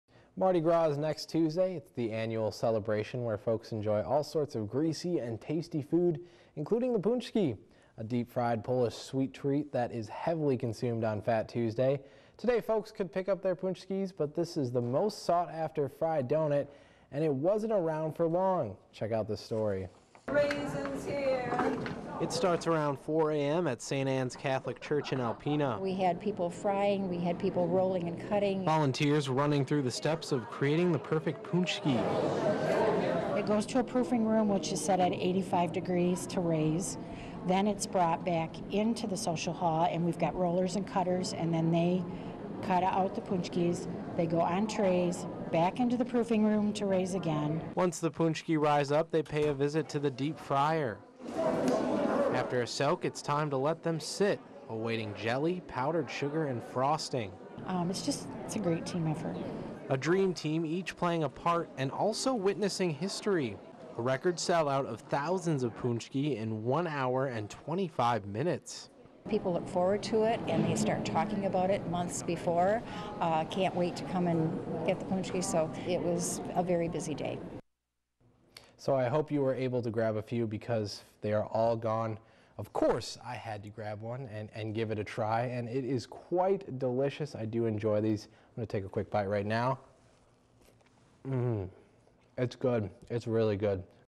Annual pączki sale sells out in record time
Alpena, Mich. ---  On Saturday, folks could pick up their pączki, but this most sought after fried donut wasn't around for long.

It started around 4:00 a.m. in the community room at St. Anne's Catholic Church.

"we had people frying. we had people rolling and cutting," said Co-chair of the event, Julie Kowalski.

Volunteers ran through steps of creating the perfect pączki. First, a fine dough was created with hundreds of pounds of flour and hundreds of eggs. Once the dough was assembled, volunteers would move it to a warm room.

"it goes to a proofing room which is set at 85 degrees to raise. Then, it's brought back into the social hall, and we've got rollers and cutters, and then, they cut out the pączki," said Co-chair Wanda Gougeon. "They go on trays, back into the proofing room to raise again."

Once the pączki rise up, they visit the deep fryer. After a soak, it's time to let them sit, awaiting jelly, powder sugar, and frosting.

"it's just a great team effort," said Gougeon.

Young and old, each volunteer has a role and plays a part. This year was special. It's the first time all of the pączki had been sold out before the 4:00 p.m. mass. It was a record hour and 25 minutes.

"people look forward to it and, they start talking about it months before, can't wait to come and get the pączki, so it was a very busy day," said Kowalski.

The money raised from the sale will benefit the All Saints Parish community as well as the Alpena community in various ways.